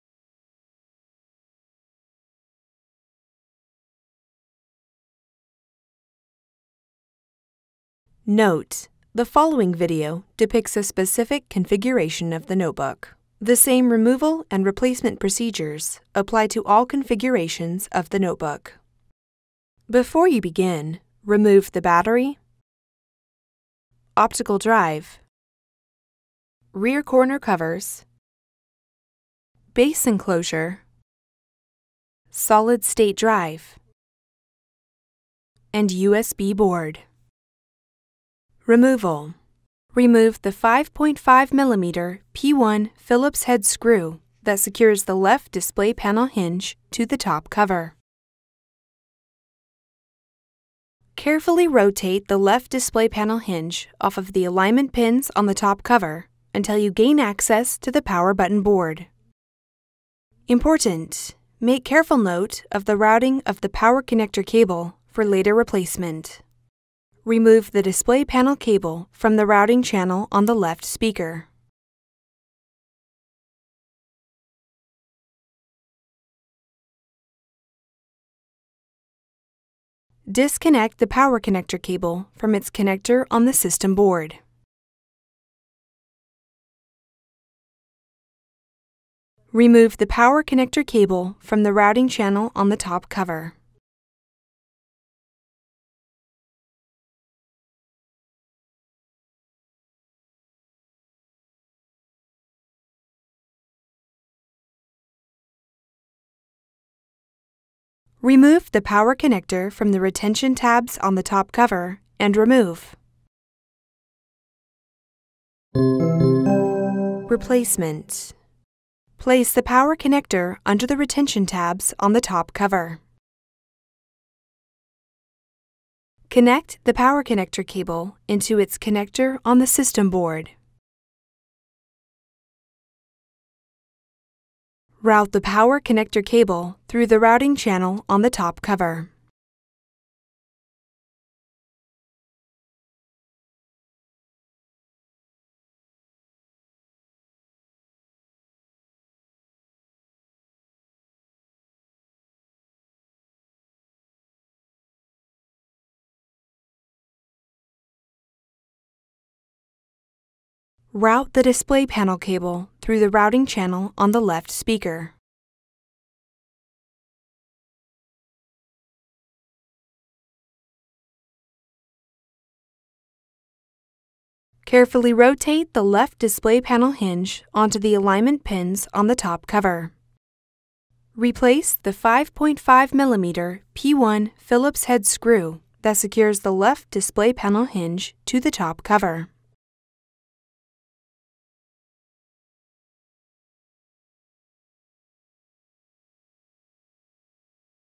Replace the Power Connector Cable | HP Pavilion Gaming Notebook | HP Support
Learn how to remove and replace the power connector cable on the HP Pavilion Gaming notebook.